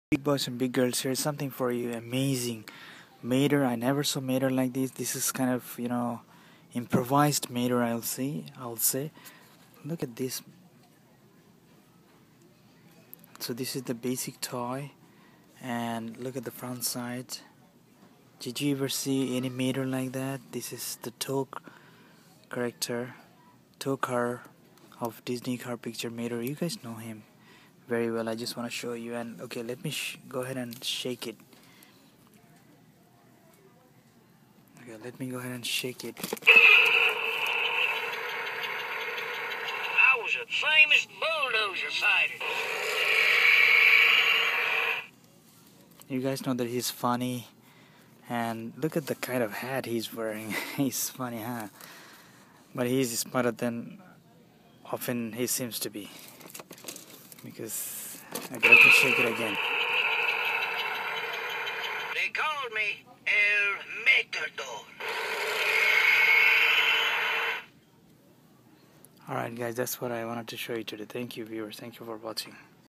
Rescue Squad Towing Car Mater - Disney Pixar Cars 2 Character with Different Look
Being a fan of Disneyland I have noticed that there are lots of disney family members to explore, Disney cars, Disney cars movie have inspired me a lot to make cars reviews videos. Here is mater of disney car and car 2 , some Disney characters have already been shot. Mater in this Video wears a brown color hat with yellow rescue chain and light sky color body. He looks funny with this look.